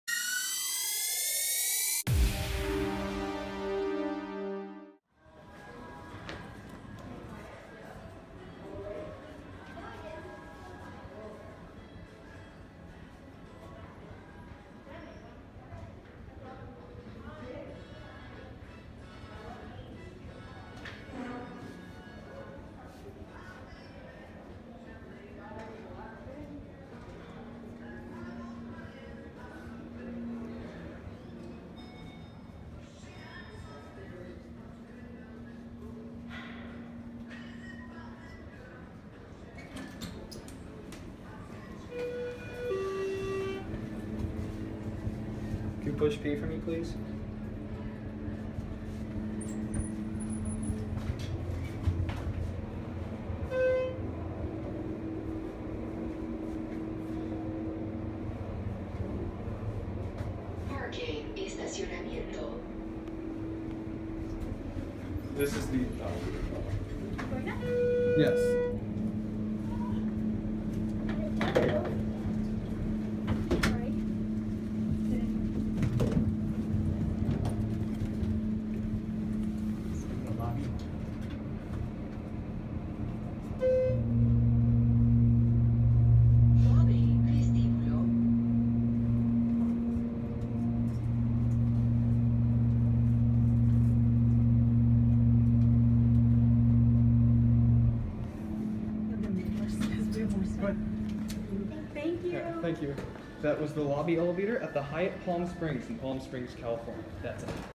Westinghouse (mod by Schindler) Hydraulic Parking Elevator @ Hyatt Palm Springs - Palm Springs, CA
This elevator was originally a Westinghouse until it was modded by Schindler with MAD fixtures. It could be an HXpress, since Schindler was offering that product for Hydraulic Elevator Modernizations while it was offering 6400(NA) for Traction Elevator Mods.

Manufacturer: Westinghouse
Modernized by: Schindler
Product: HXpress
Motor Type: Hydraulic
Fixtures: MAD